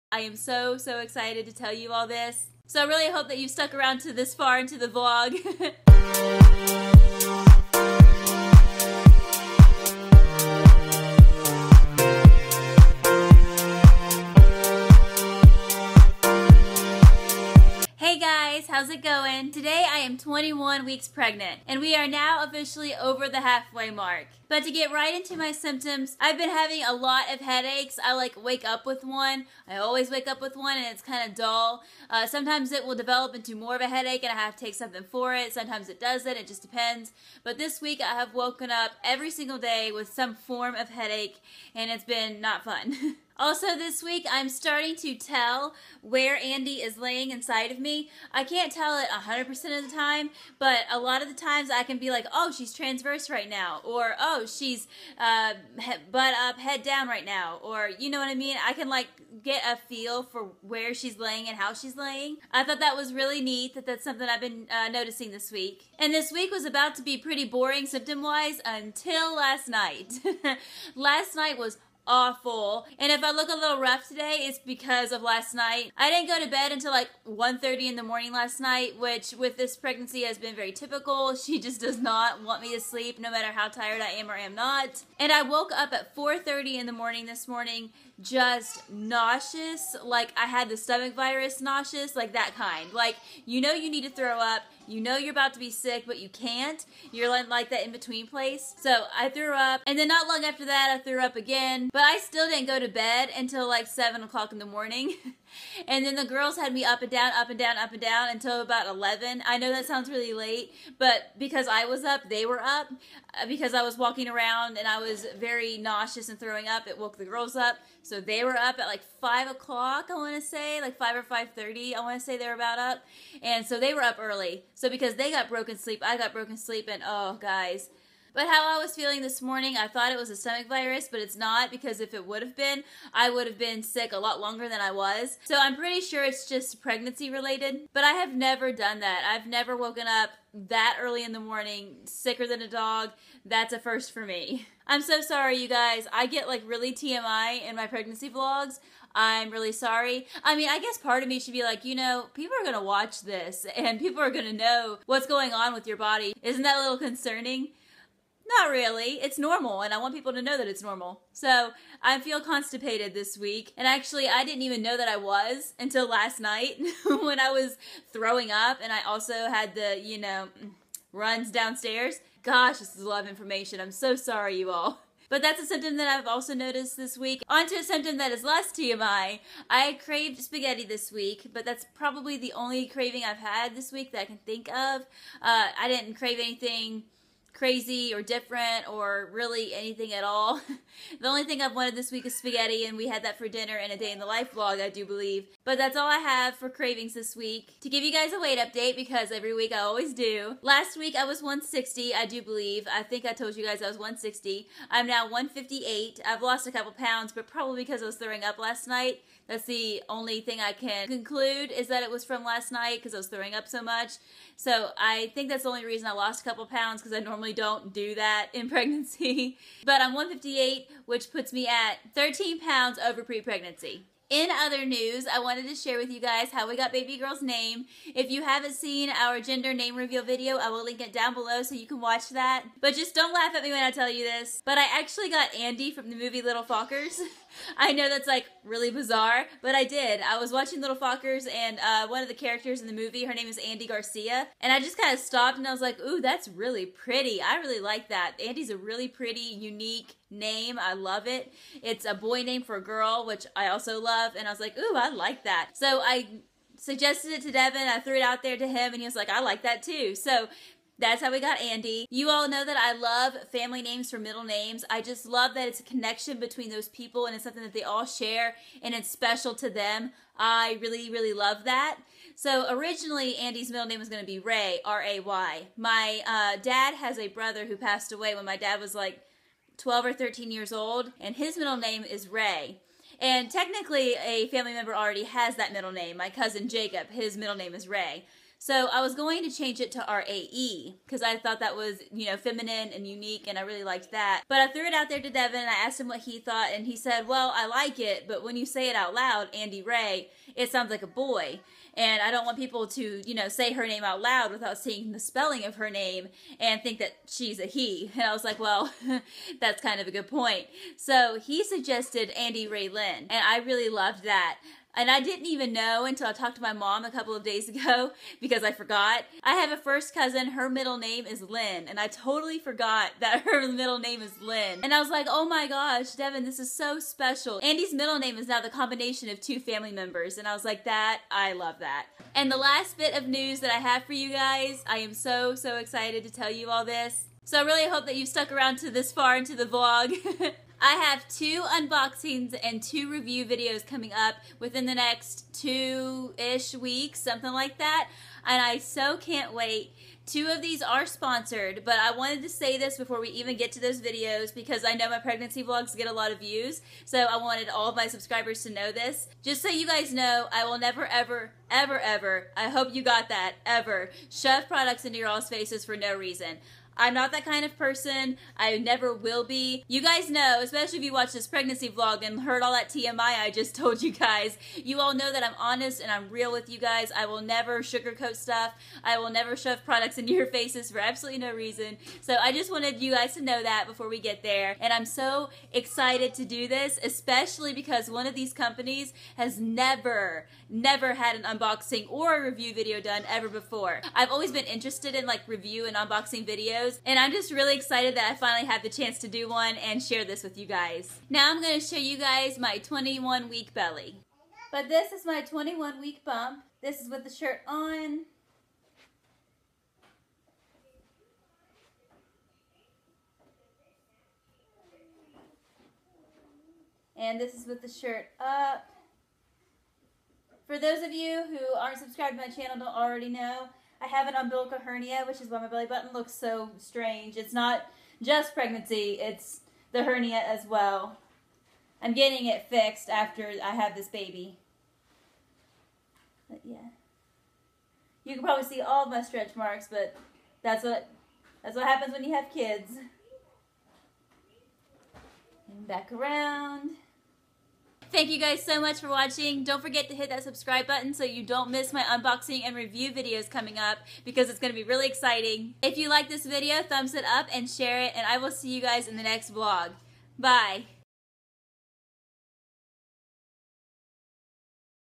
HOW DID WE GET HER NAME? | 21 WEEKS PREGNANT
~~OPEN ME~~

Hey guys! Today I am 21 weeks pregnant and man did I have a bad night. I also get a little TMI with you and share some exciting news!

~~SOCIAL MEDIA~~